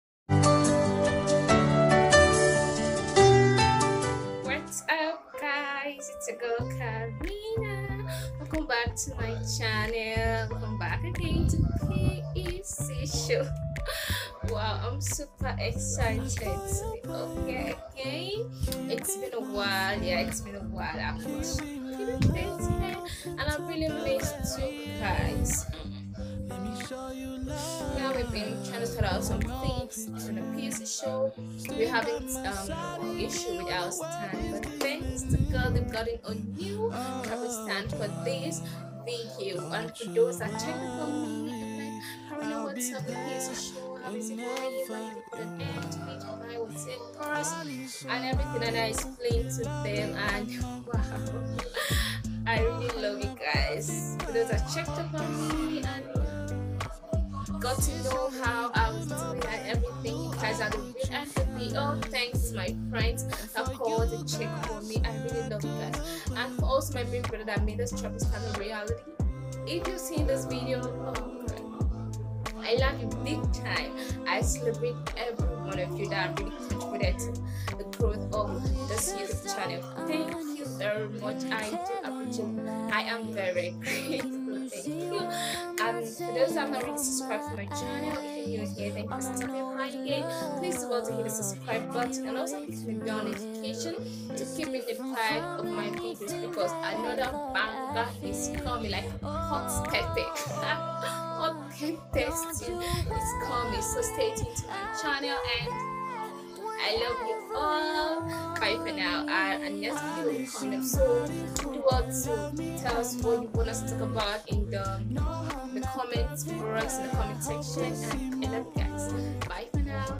WHY I STOPPED THE PEC Show
Hello family welcome back again to my channel

I filmed this video to let you know why I stopped the PEC Show,the reasons behind it.

Finally we're back to continue from where we stopped.